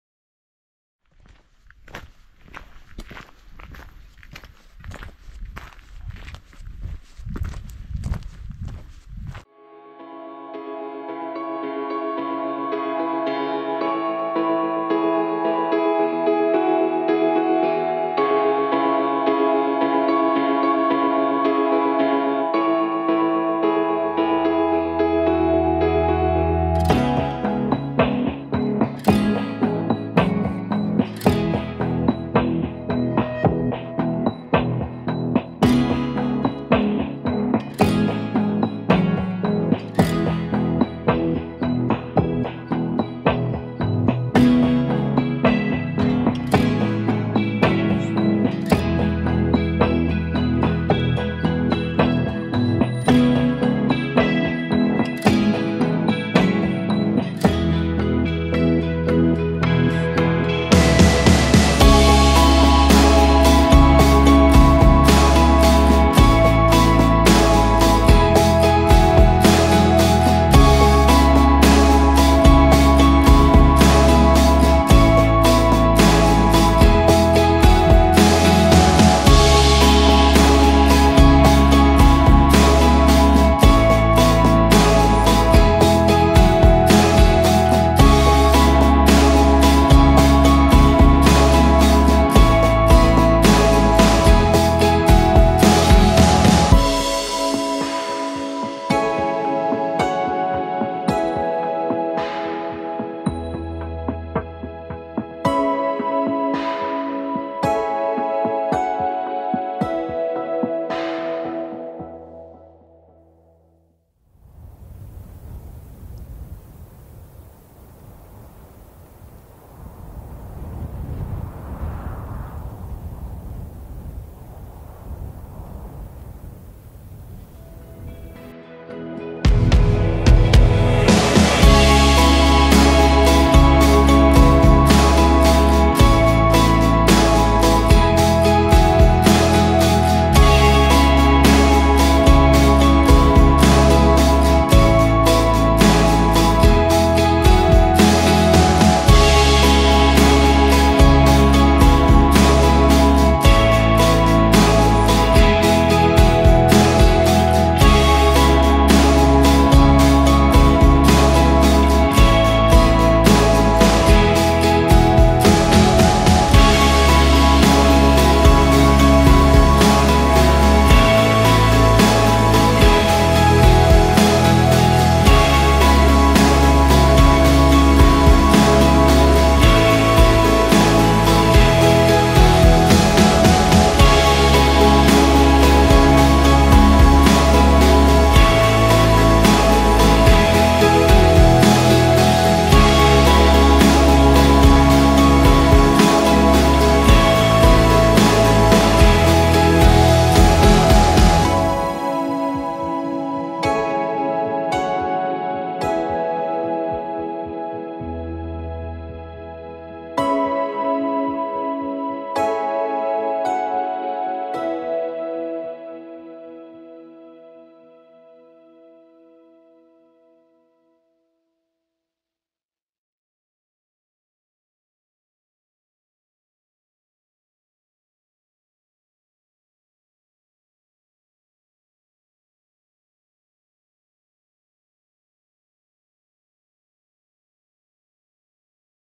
L'affaccio sulle Gole della fiumara La Verde da contrada Giulia
Non ci sono parole per descrivere la bellezza delle gole della Fiumara La Verde con le sue alte pareti rocciose a strapiombo che nel punto più stretto sembrano toccarsi. Per poterle filmare e fotografare dall'alto abbiamo raggiunto l'affaccio di contrada Giulia con una breve passeggiata su un percorso ad anello che offre panoramiche straordinarie su tutta l'area della fiumara La Verde.